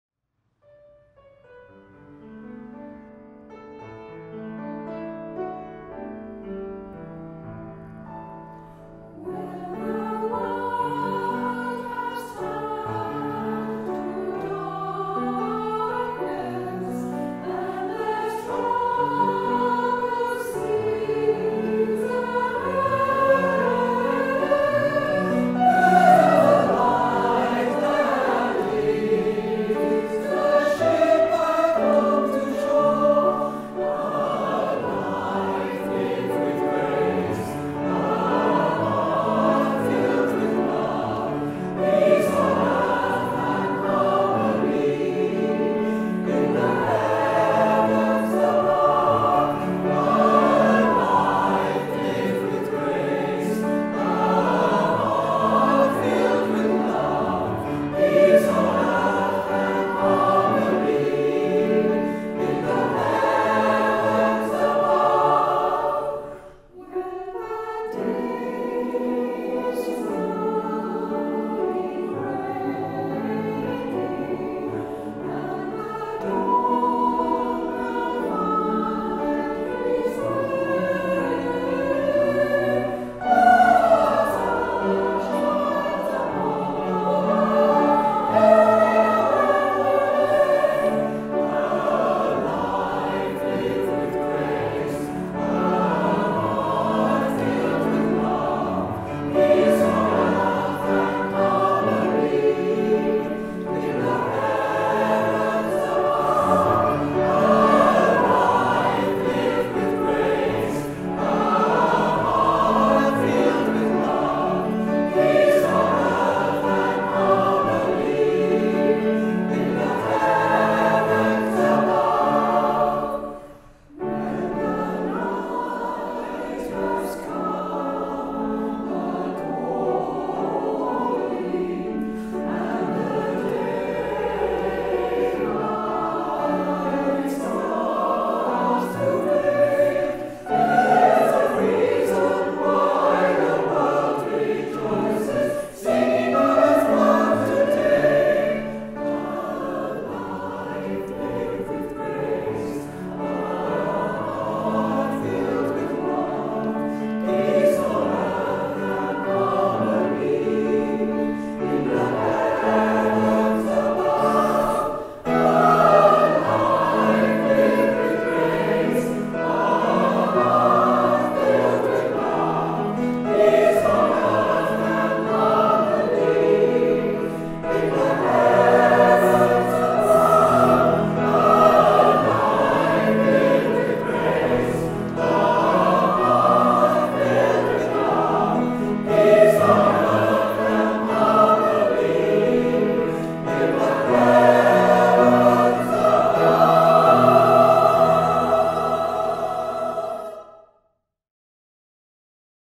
Taunton choral society
Taunton Choral Society have kindly sent us this lovely video of their final rehearsal before their performance at the Queen's Platinum Jubilee celebrations on Thursday!